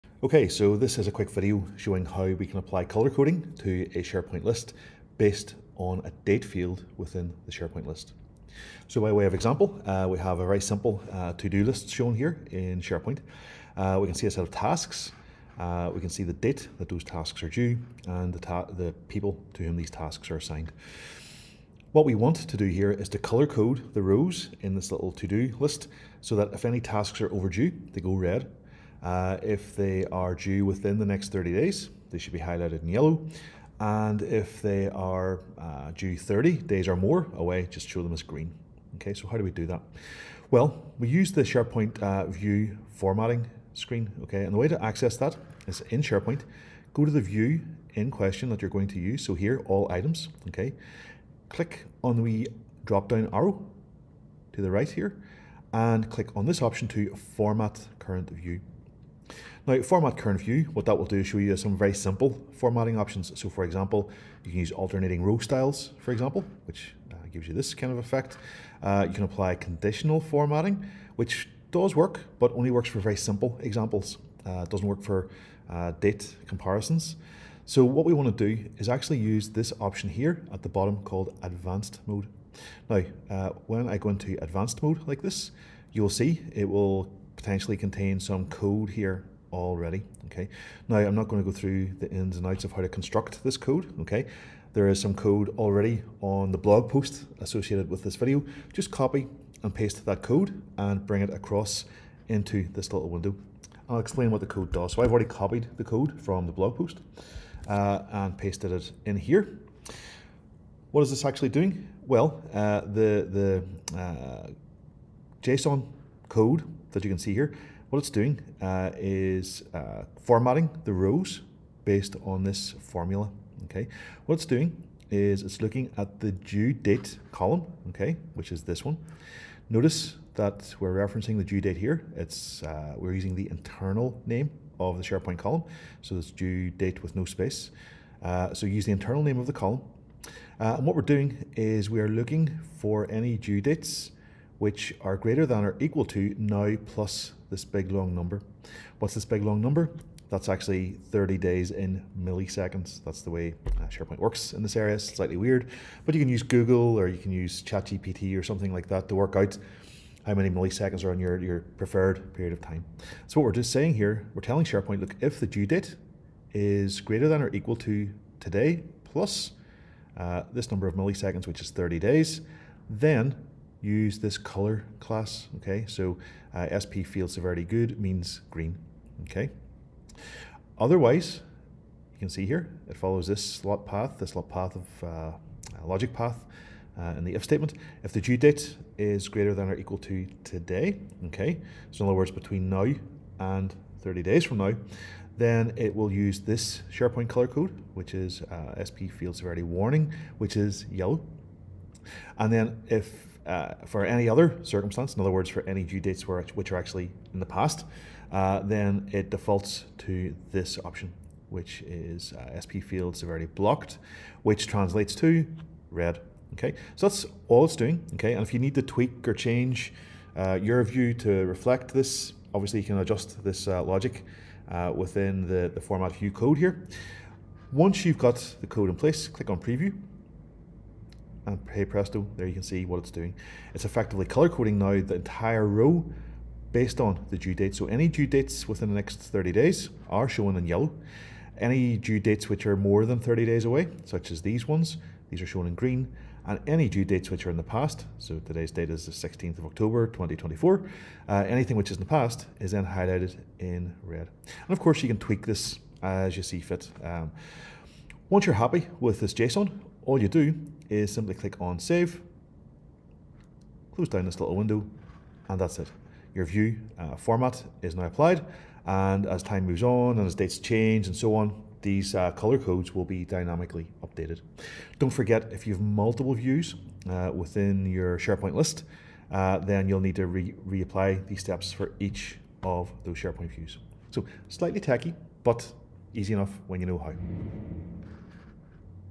Advanced Color-Coding for SharePoint Lists
SharePoint lists are great!  They are brilliant for many users, allowing much better storage of structured data than Excel spreadsheets.

You can also do simple color coding on SharePoint lists based on simple field values.  But a common request I hear from customers is:

How do we color-code the list based on a date column in the SharePoint list?

So for example, if a To Do list in SharePoint has a field called "Due Date", how can we color code it so that rows are green if the due date is more than 30 days away, yellow if the due date is within 30 days, and red if the due date is in the past?

It's not at all clear how to do this in the SharePoint View formatting screen, but luckily there is a way to do it!

See how in this little video, and see the Blog Post linked below for the code snippet used.  Slightly tricky, but easy when you know how!  🤓

Questions?  Give me a shout !